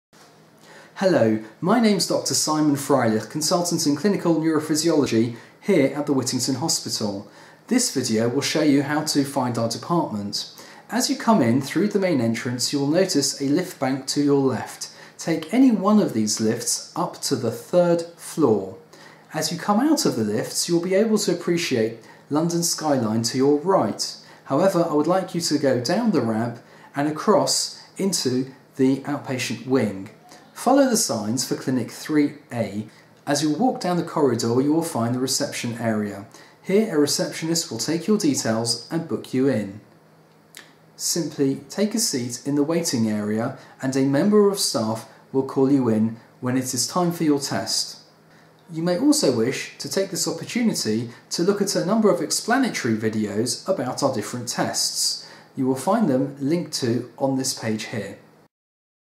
Whittington Health Neurophysiology Department
A warm welcome to the Department of Clinical Neurophysiology at the Whittington Hospital! This video will show you how to find our department. It also provides links to our explanatory videos regarding the tests we perform. These might not be available on all mobile phone platforms.